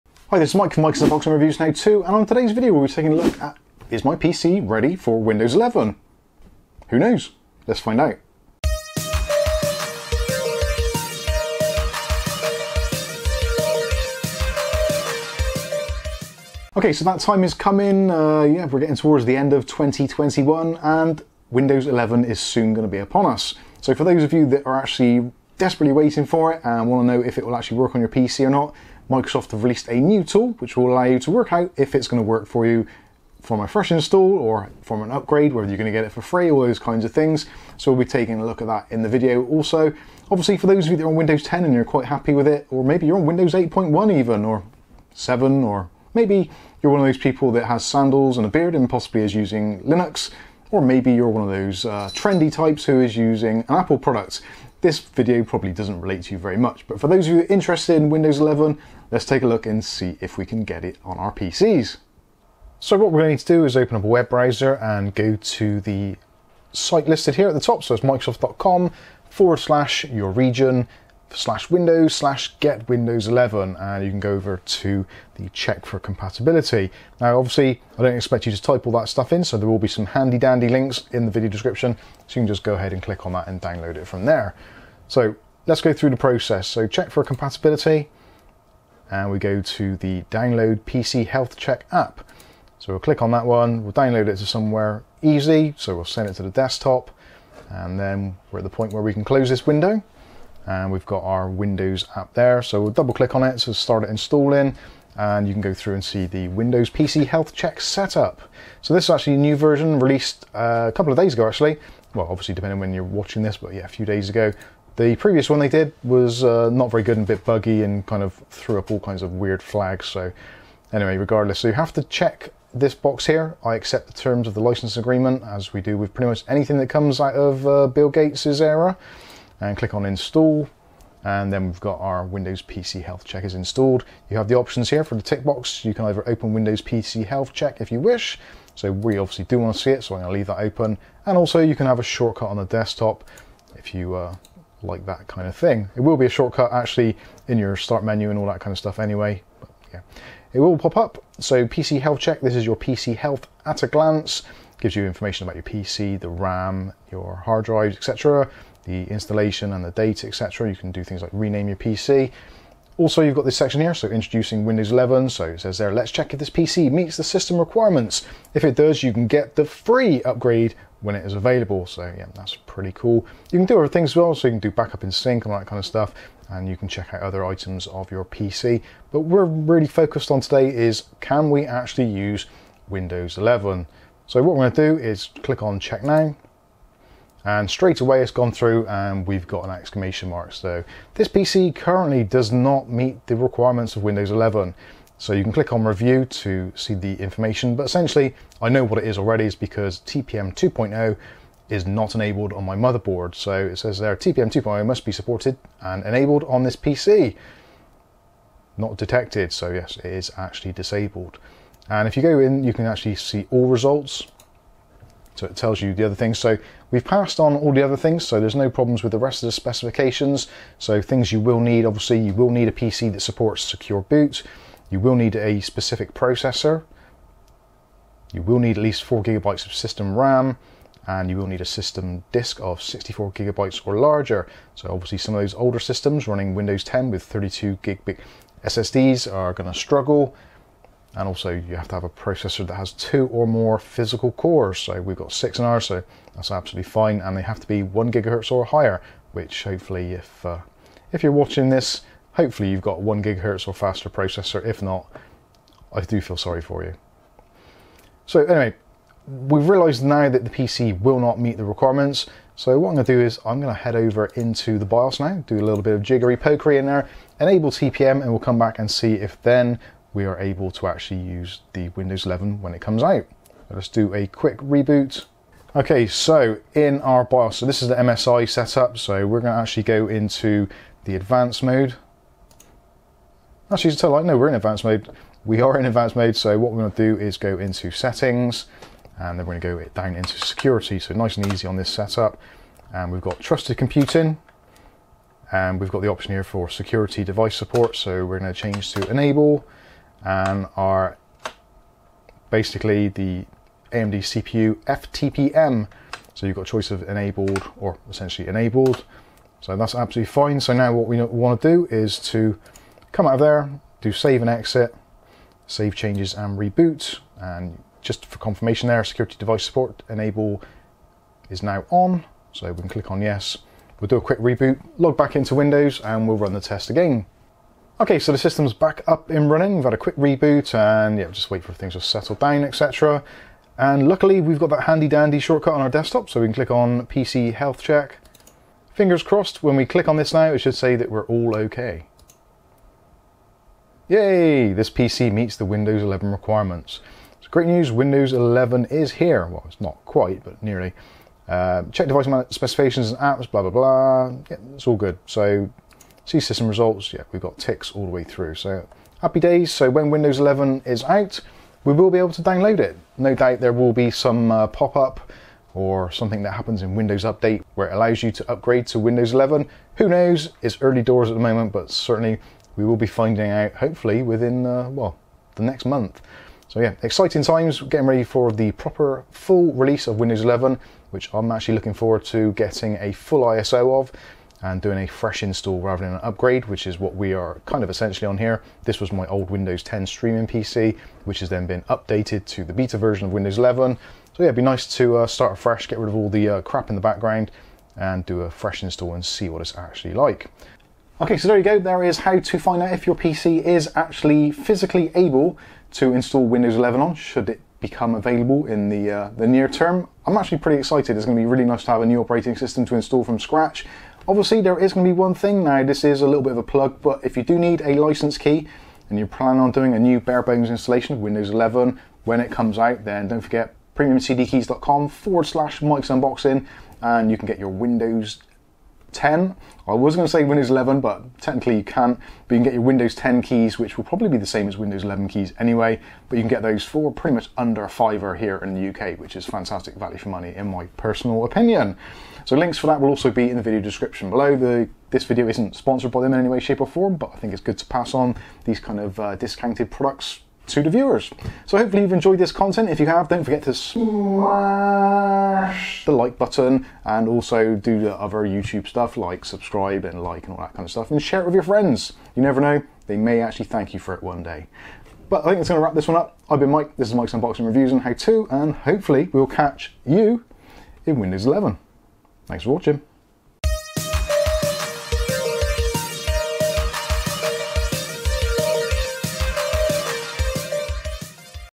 Can YOU Get Windows 11 FREE & How To Check
Can YOU get WIndows 11 FREE?  & how to check if your PC is compatible!

Windows 11 is going to be the BIG thing in a few weeks, and is looking to be a free upgrade, and most likely will be able to be installed using an existing or purchased WIndows 10 license key, that is great but is YOUR PC able to actually install and run it?

In the video we will be using the Microsoft PC health check app to find out, YOU might want to do the same before attempting to install the upgrade or a FRESH install to prevent any issues during the install.